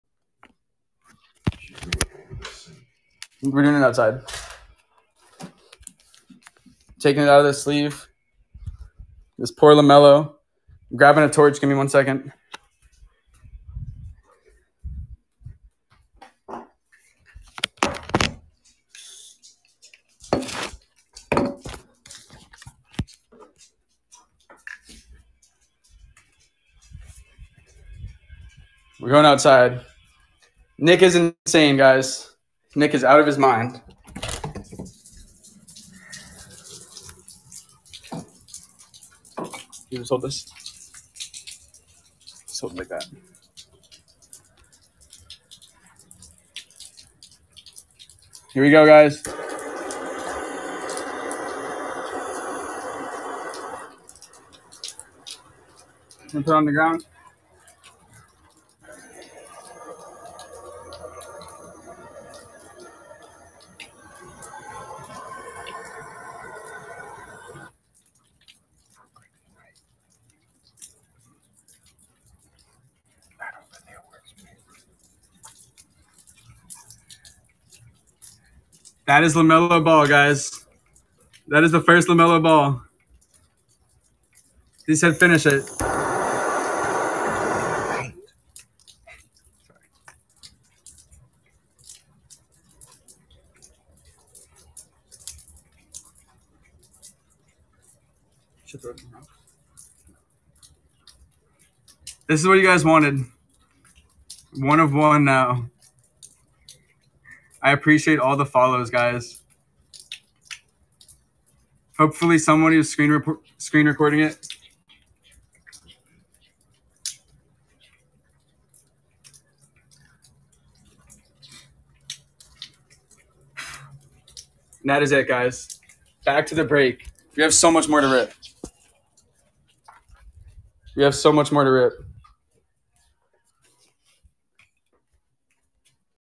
LaMelo Ball Prizm RC Burn
nickanzalone the legend pays to burn a rookie LaMelo Ball Panini Prizm Card